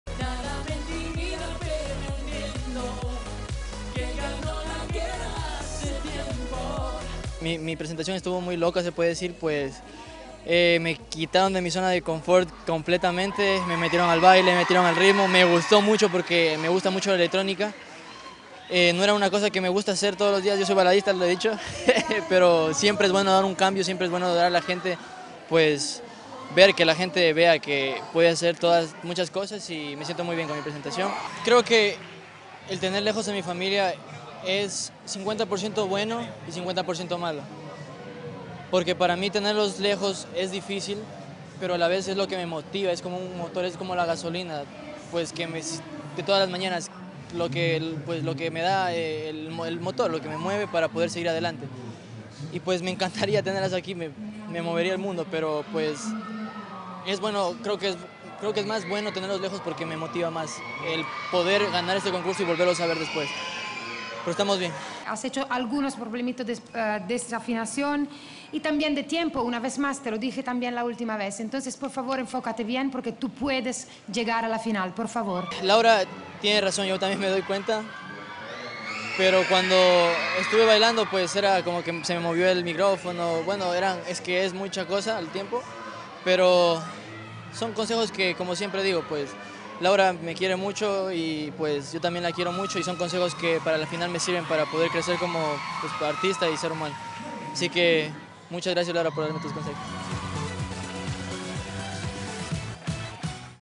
Christopher está agradecido de las críticas de Laura
El participante ecuatoriano quedó conforme con su presentación a pesar de las críticas del jurado.